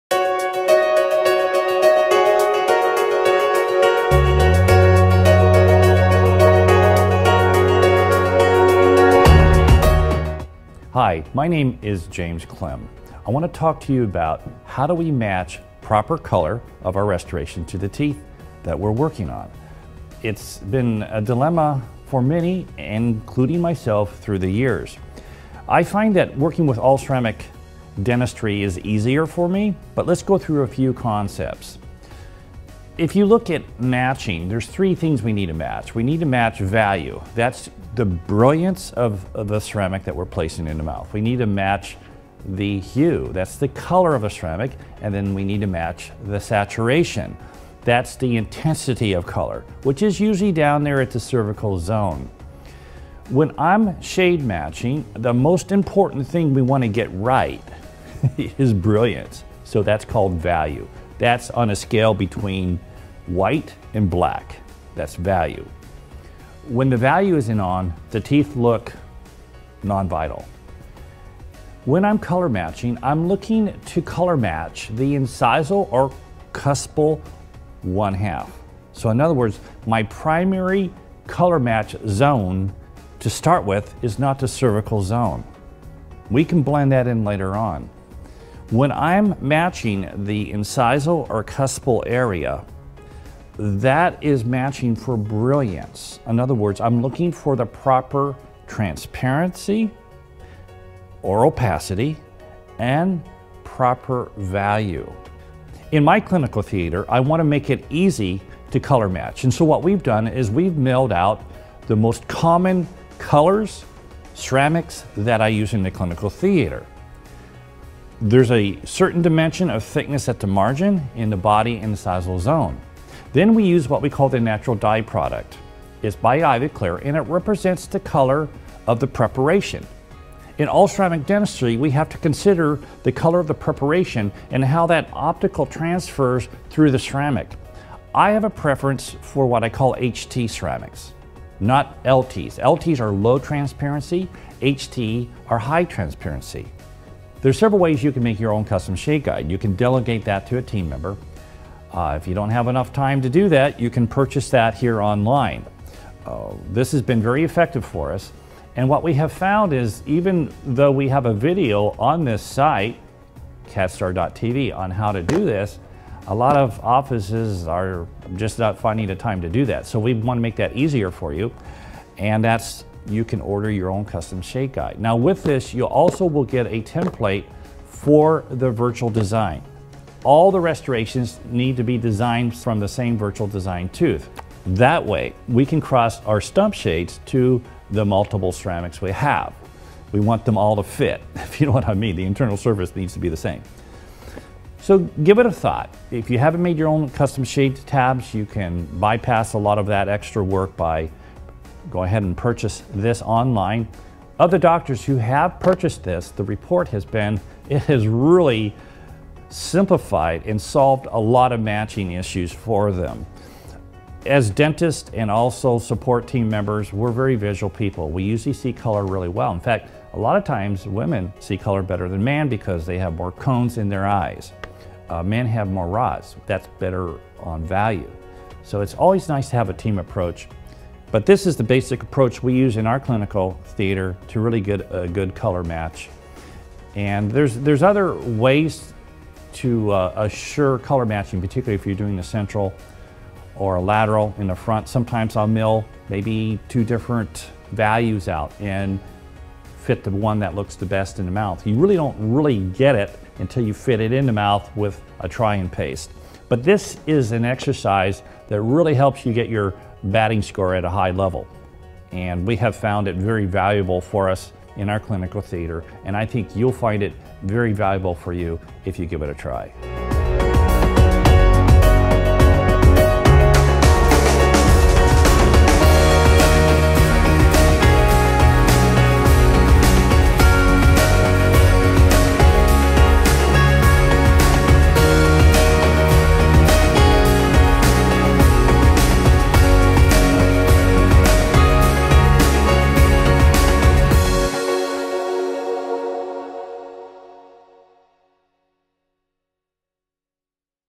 CEREC Custom Shade Guide
Dr. Klim's Custom Shade Guide
Now available for international orders through eBay
Assists in color matching CEREC restorations. Use the pre-modeled stump shades and restorations to predict final color. Choose the stump shade, place the different crowns on top and choose the shade that best matches the patients existing color.